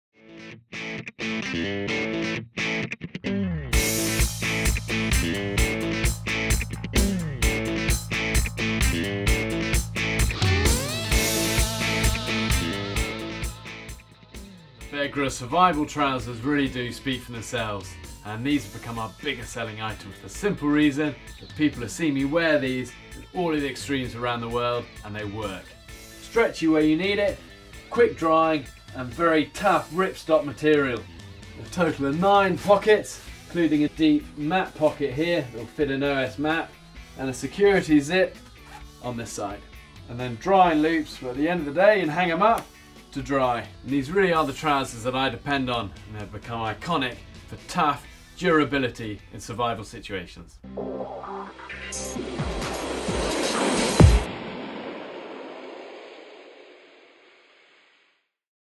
Bear Grylls Trousers - Official Demo
Bear shows us the best features from the Survival Clothing Range trousers. It'll take more than a week on the trail to get the better of these technical tigers. Primed with all the performance features you'd expect and agile enough to keep up when you're on the move. Get out and give them a run for your money!

"Stretchy where is counts and when it matters, these trousers take a beating yet endure and endure." Bear Grylls